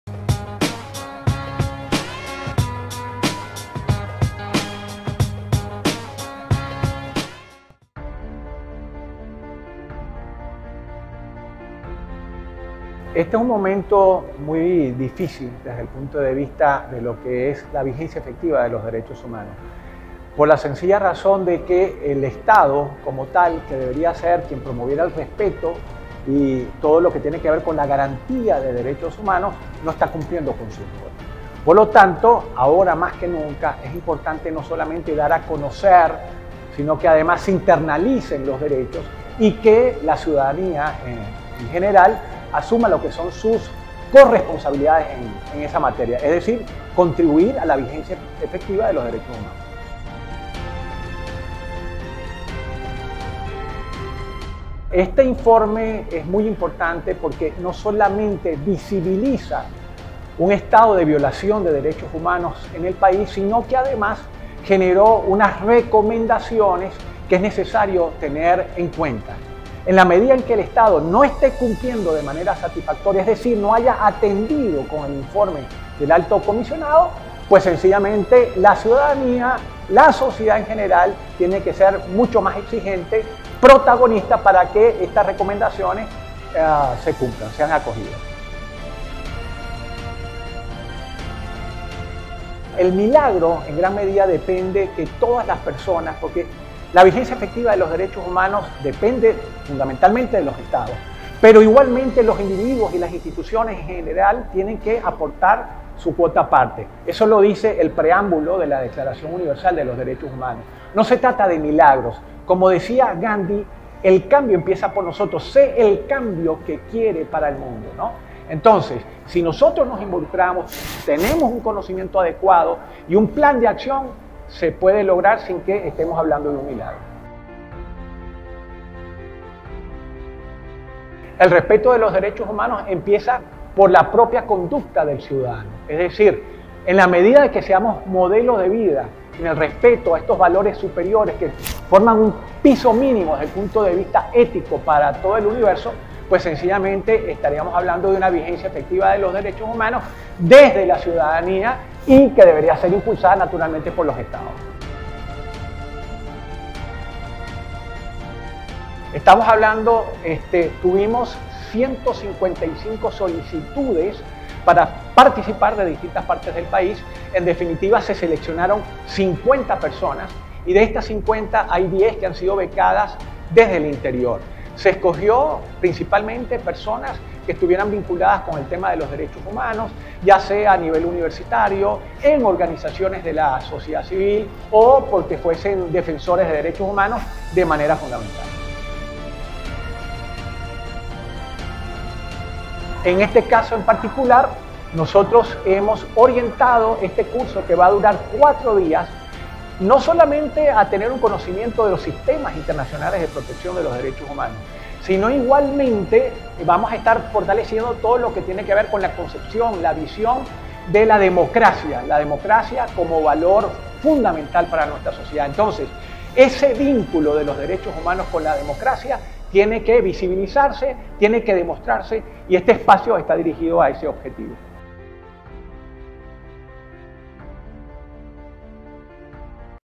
CDH UCAB: Ciudadanos deben asumir su rol y exigir vigencia de DDHH
Durante la instalación del XXI Curso sobre Sistemas Internacionales de Protección de Derechos Humanos, el coordinador de educación del Centro de Derechos Humanos de la UCAB, José Bernardo Guevara, aseguró que no basta visibilizar y denunciar la violación de las garantías fundamentales, sino que la población debe formarse y organizarse para exigir su vigencia